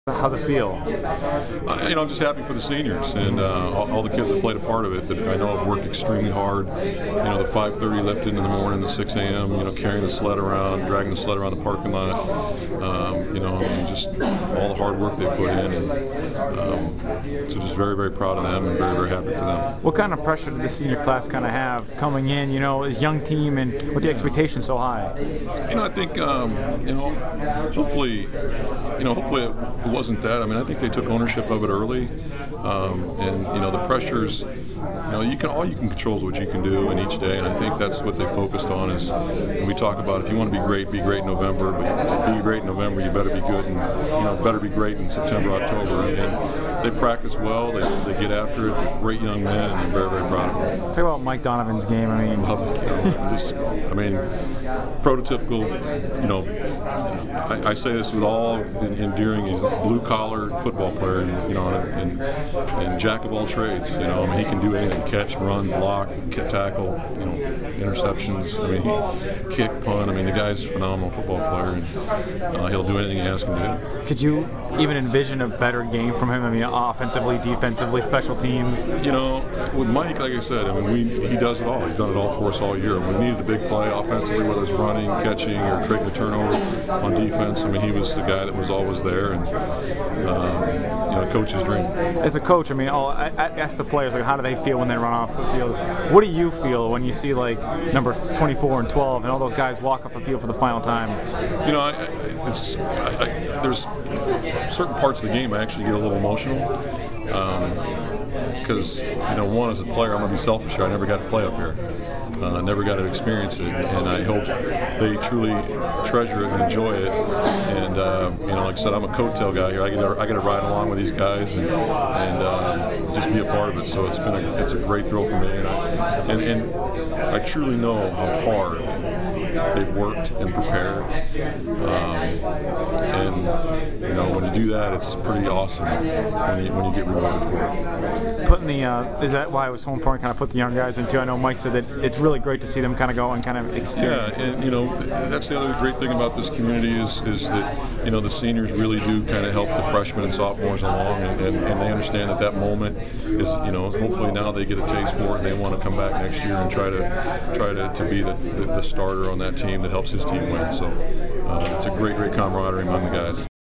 Regina head coach Marv Cook state title postgame
Regina head coach Marv Cook talks about his team's third straight state championship after a 35-14 win over St. Albert on Nov. 16, 2012.